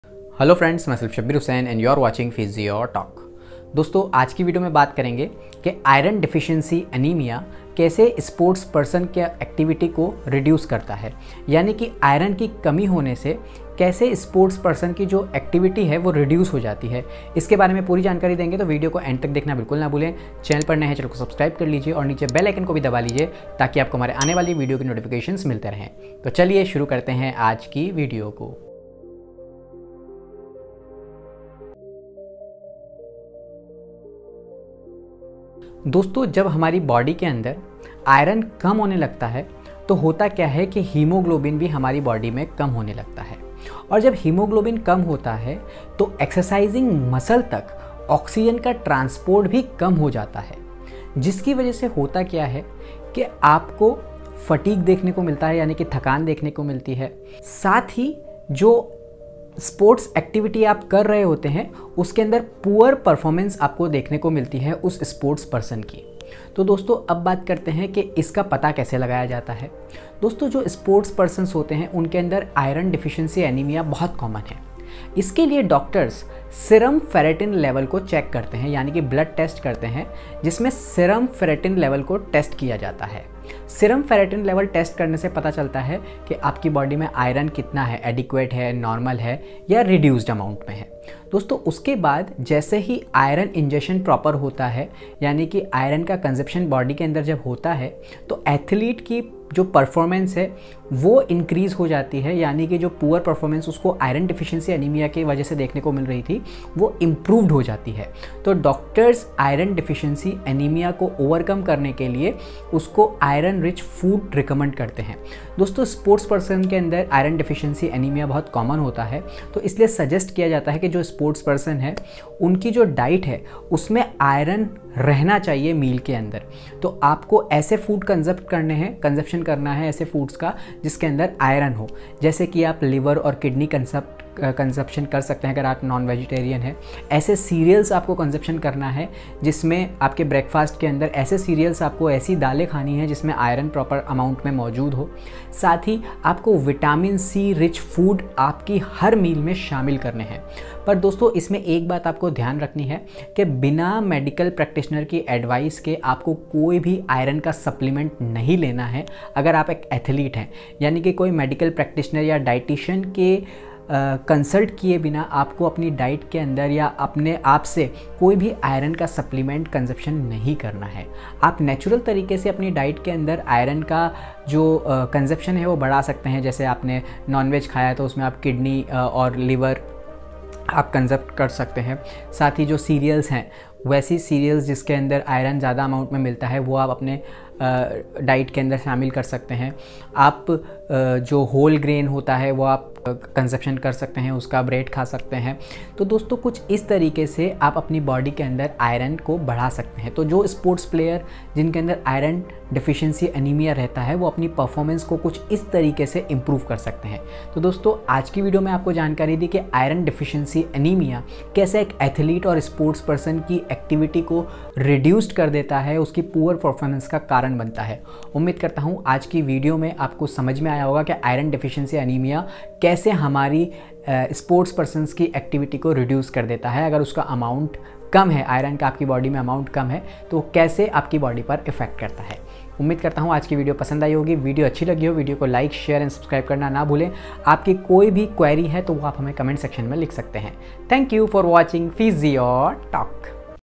Effect of iron deficiency on athlete performance | iron deficiency anemia
Having an iron deficiency, or in severe cases, anemia, can be detrimental to athletic performance and overall health. It limits the body's capacity to carry and deliver oxygen, thus stunting potential maximal oxygen uptake (VO2max), or work capacity.
Physio talk Amazon store
HEAT MASSAGER TO RELIEVE SHOULDER PAIN BUY posture support belt TO RELIEVE PAIN BUY
Chapters
00:00 - Effect of iron deficiency on athlete performance
00:29 - Physio talk intro
00:36 - iron deficiency anemia
00:56 - symptoms of iron deficiency
01:17 - test to identify iron deficiency
02:00 - Treatment of iron deficiency anemia
02:57 - foods rich in iron
03:30 - conclusion